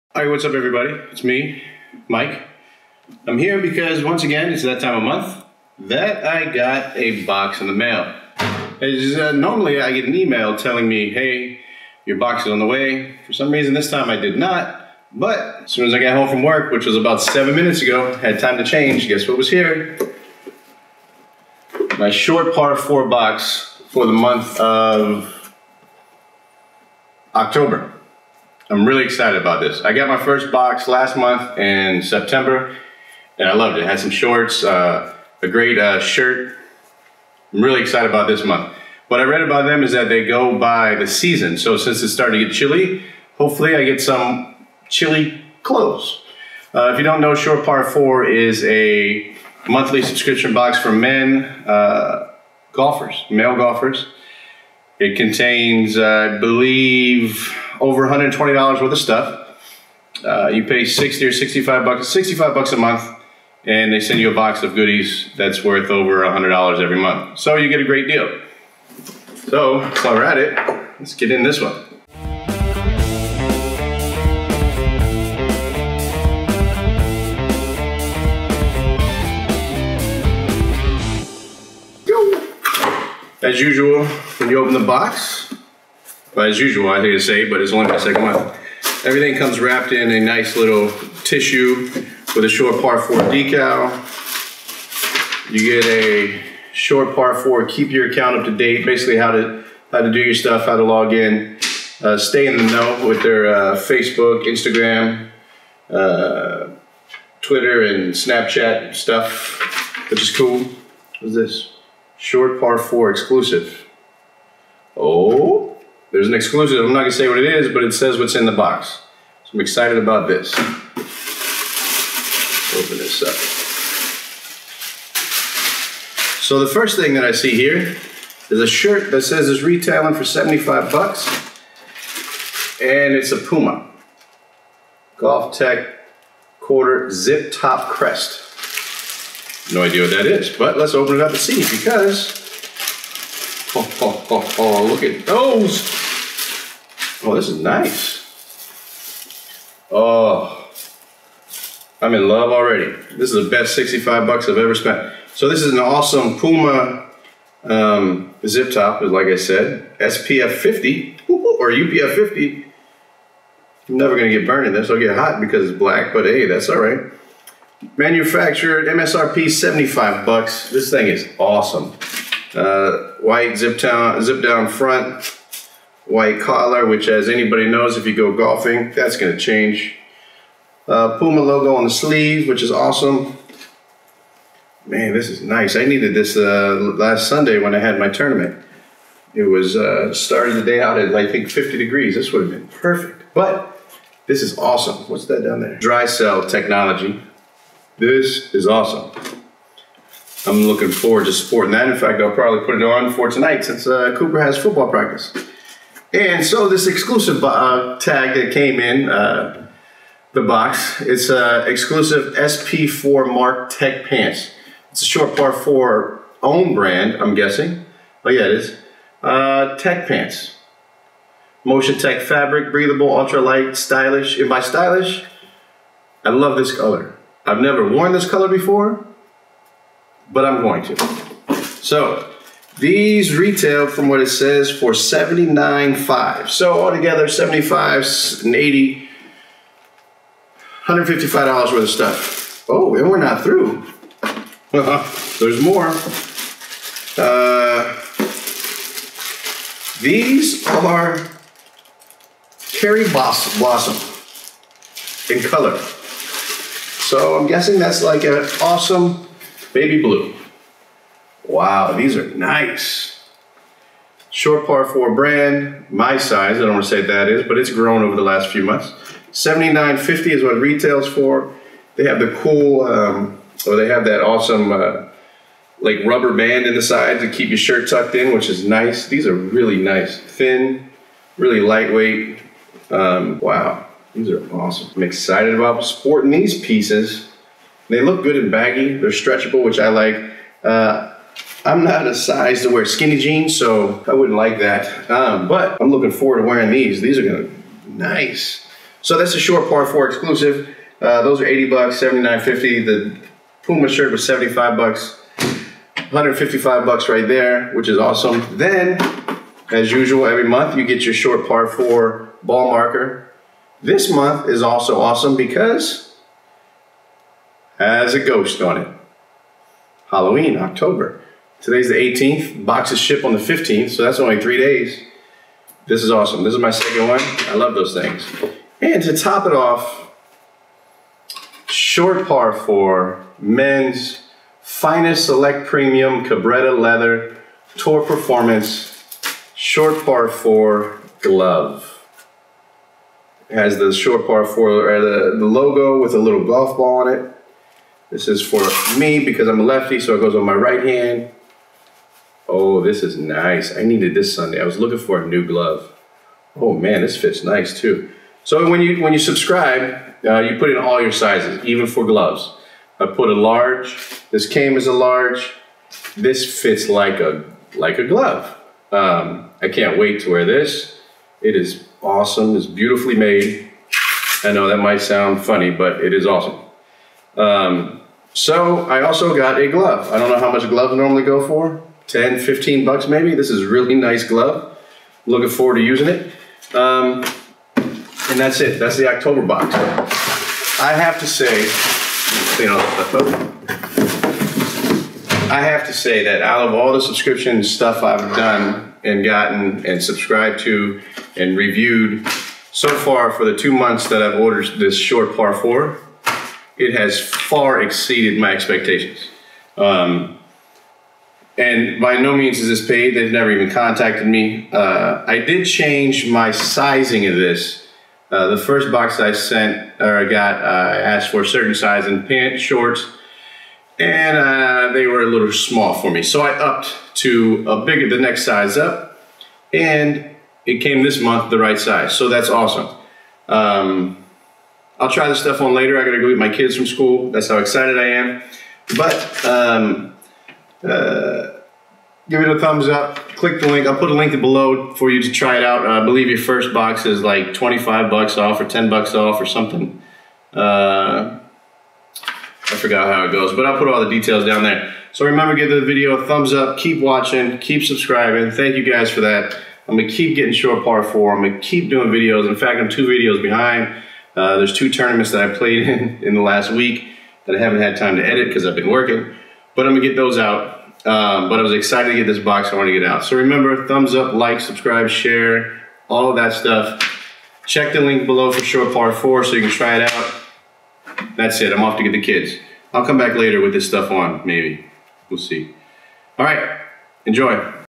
Short Par 4 October Unboxing | SP4 is AWESOME!!
(sorry for the blurry image...)

BOY OH BOY!!! The October Short Par 4 box to me was the best so far!!!

In this box was 3 things that I really could use and was actually thinking of buying anyways.

Starting off this box was a Puma Golf pull-over. It is AWESOME!! The tag has an MSRP of $75.

Next up was a SP4 exclusive! A pair of pants that I have been needing for a long time. They have an MSRP of $79.50 and finally there was a custom SP4 golf glove!

All together this box should have retailed for over $160 according to the tags and I got it for only $45 bucks...

I made a mistake in the video and said the box was $60 but I was mistaken. It is ONLY $45!! But with the link below you can save even more when you sign up.

Remember to head on over to Instagram also and give us a follow!